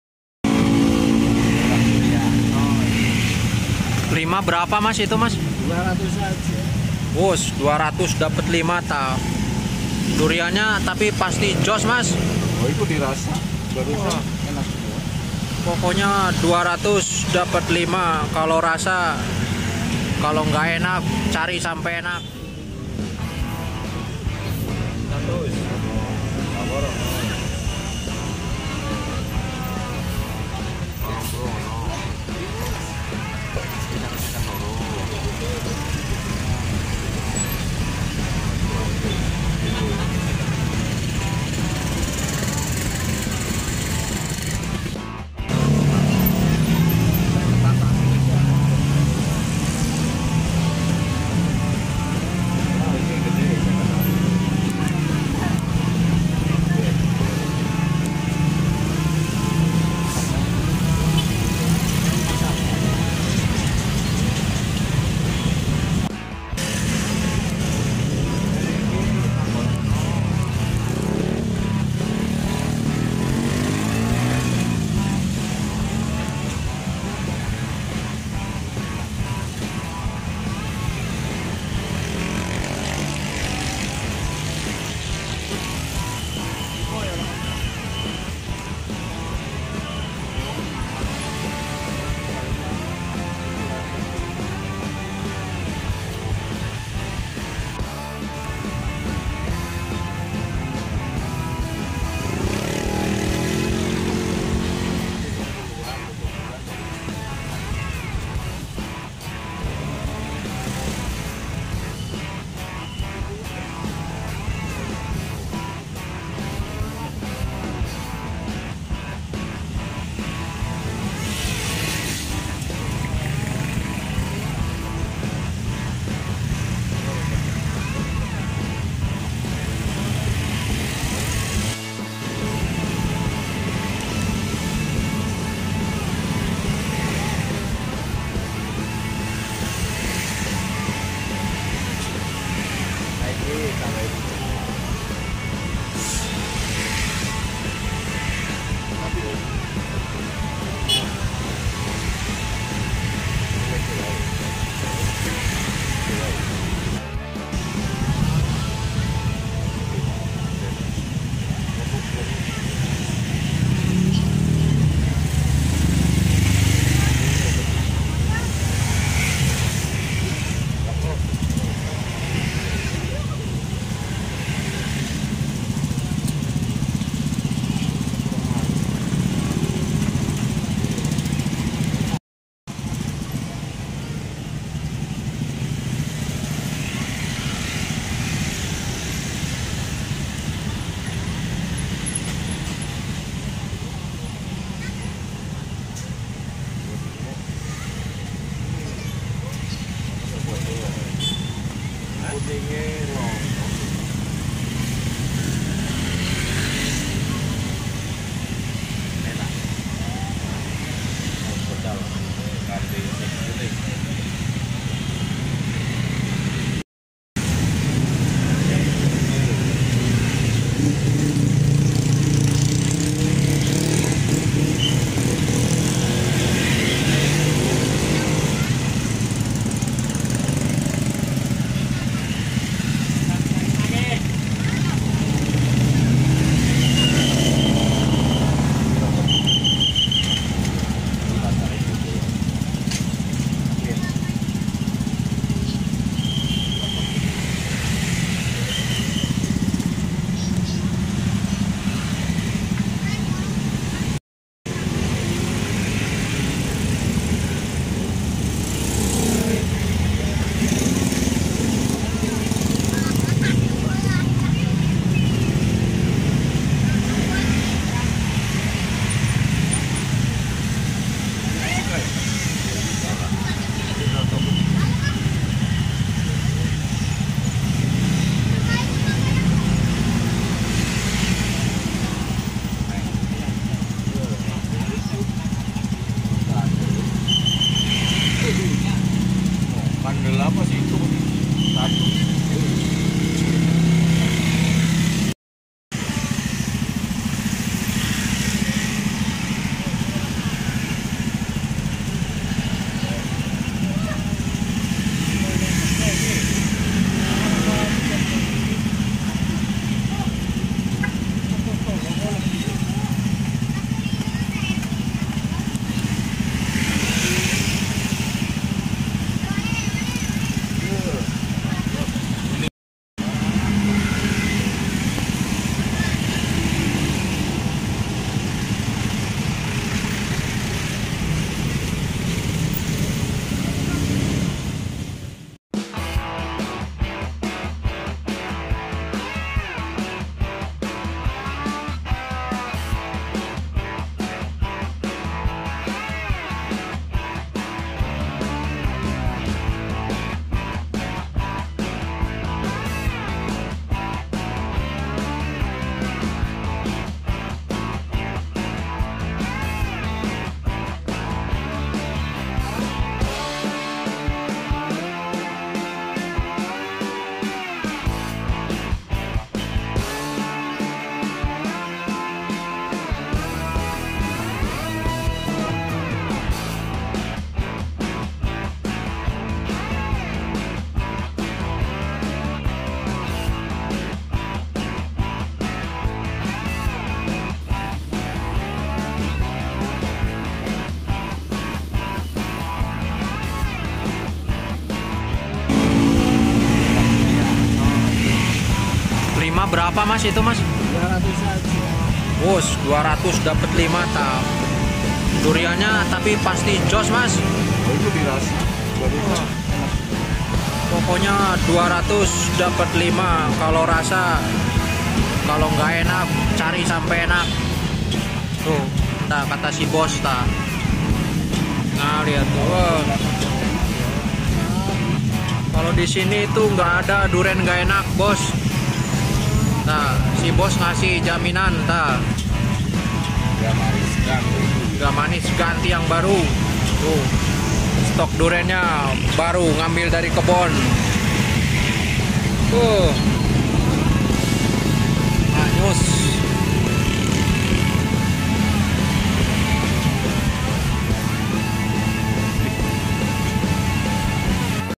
DURIAN MURAH , contoh penjual durian jujur , pembeli selalu icip sebelum di bungkus !
jl Cikutra depan TMP pahlawan bandung

merapat